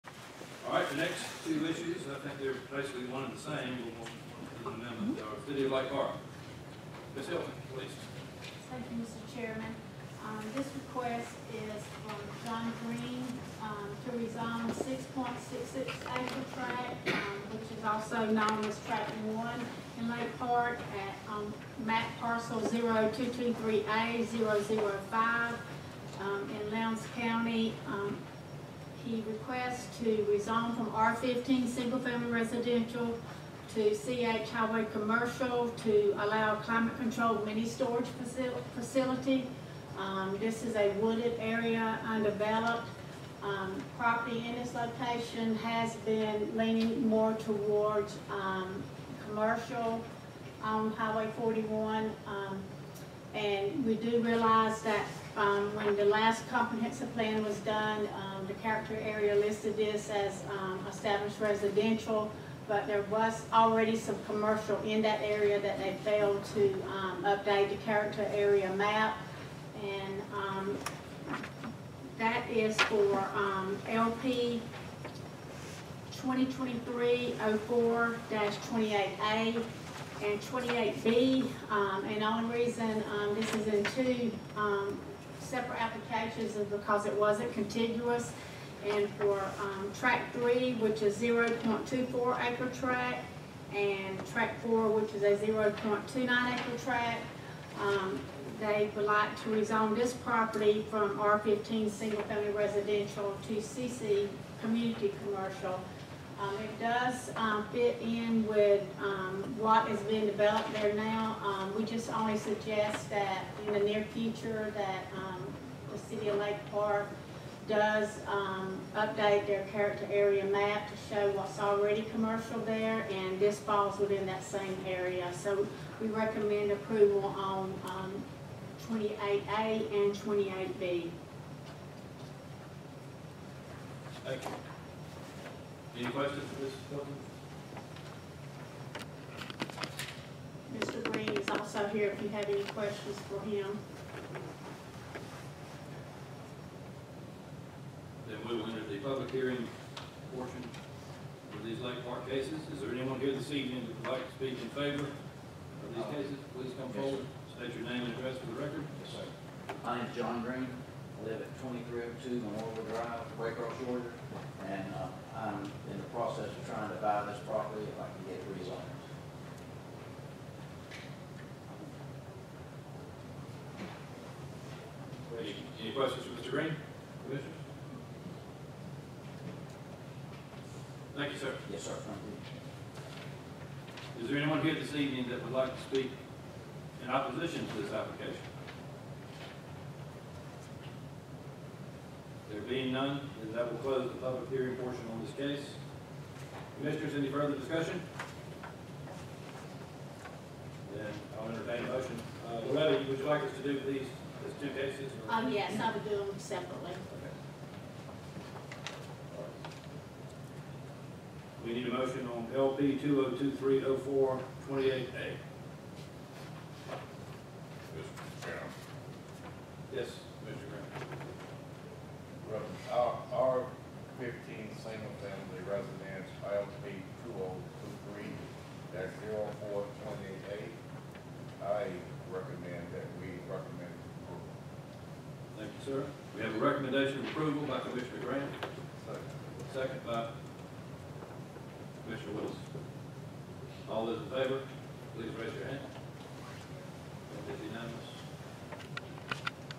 5. LP-2023-04-28A Cody Califf, US Hwy 41 R-15 to C-H @ GLPC - 2023-06-26
5. LP-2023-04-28A Cody Califf, US Hwy 41, 0223A 005 (Tract 1), 6.66 acres, Current Zoning: R-15 (Single-Family Residential)
Proposed Zoning: C-H (Highway Commercial)